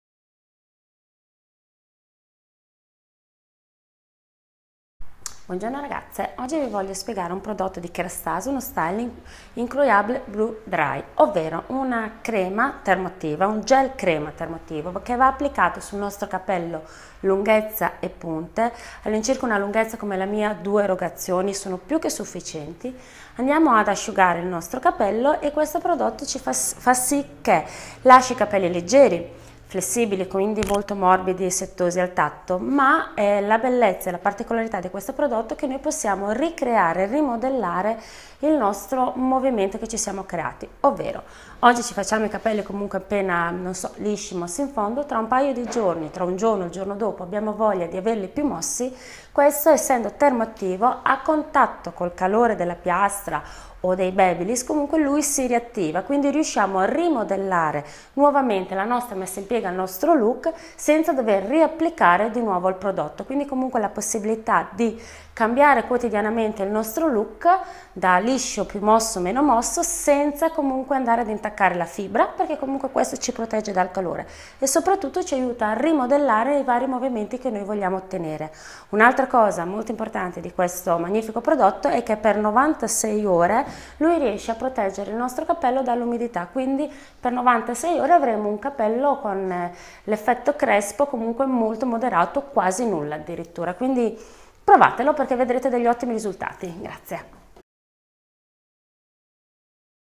Tutorial Incroyable Blowdry Spray Kérastase
Maurizia Hair Stylist di Reggio Emilia ci consiglia come utilizzare al meglio lo spray Incroyable Blowdry di Kérastase.
Potete trovare questo prodotto e tanti altri a prezzi vantaggiosi qui